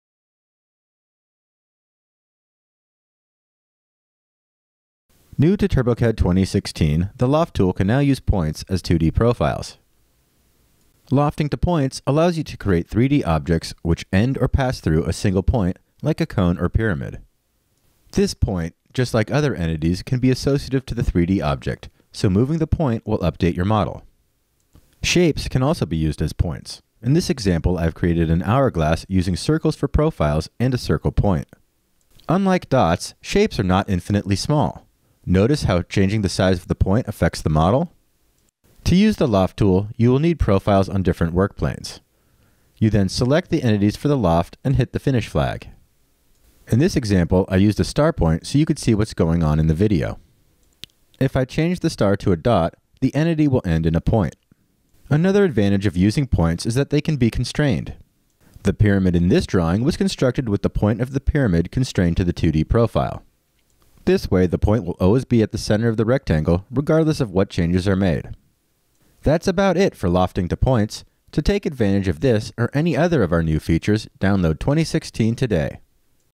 Lofting to Points
This video shows new option of the loft tool.